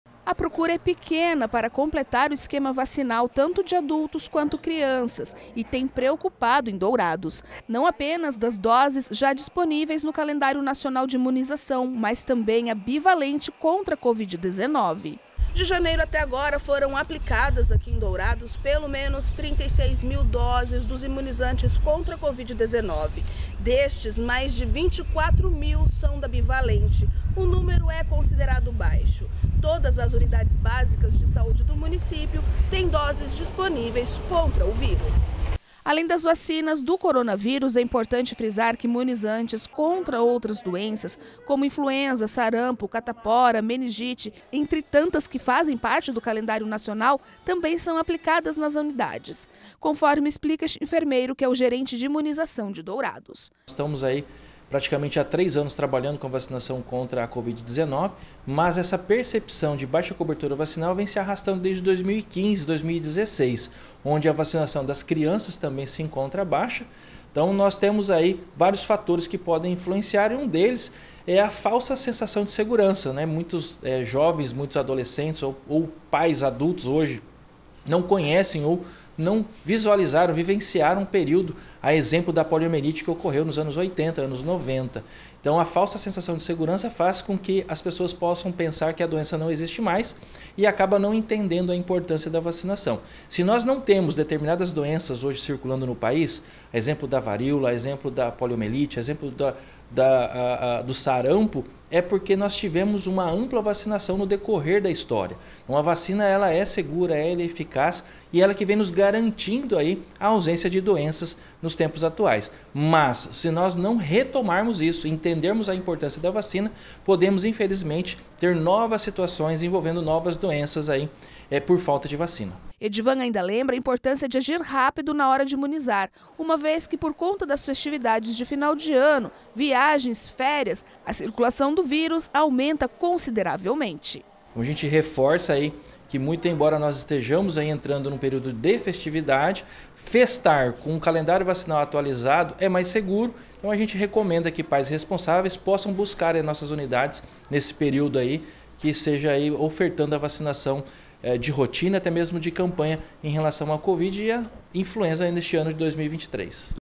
Dourados registra baixa procura para completar o esquema vacinal de Influenza e Covid-19
De janeiro até agora foram aplicadas em Dourados em torno de 36 mil  doses de imunizantes contra a Covid 19. Dessas 24 são bivalentes,  número considerado muito baixo. As doses estão disponíveis em todas as unidades básicas de saúde do município não do esquema vacial de Influenza e Covid-19, como também os demais imunizantes para prevenir outras doenças.